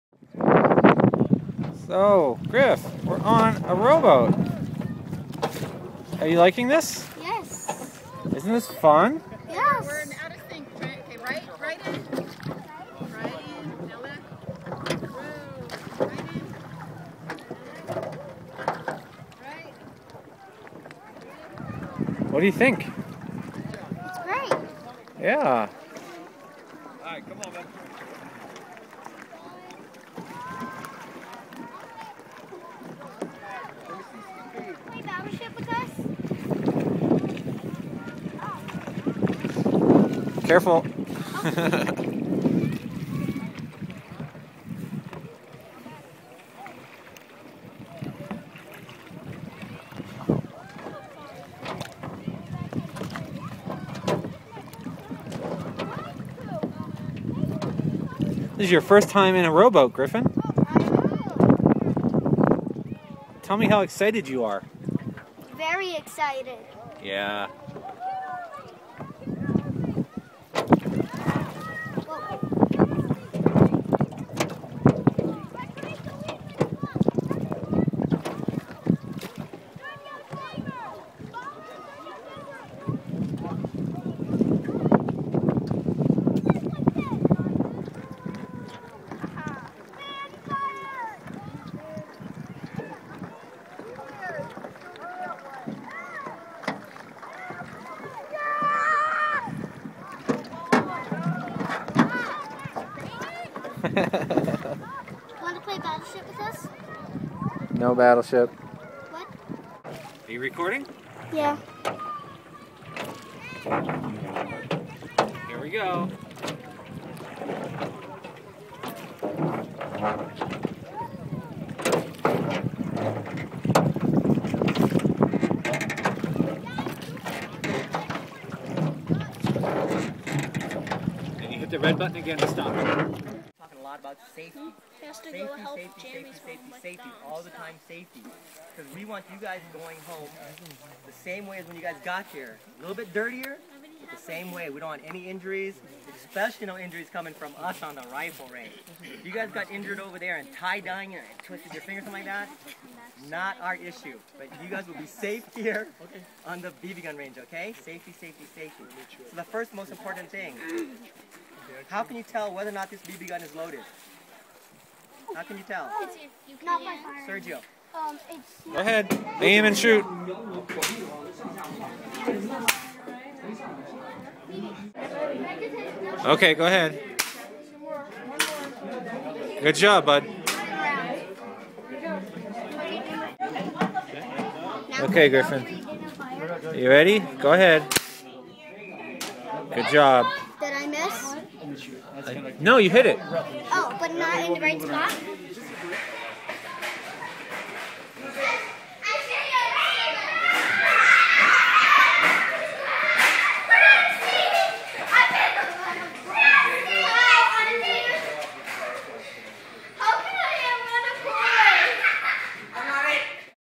Camp Whitsett-Day 2-Row Boats & BB Gun.mov
Griffin's first row boat and his most anticipated activity, BB gun shooting.  HD video available by choosing 720p instead of 320p.  Click bottom right corner where you see 320p.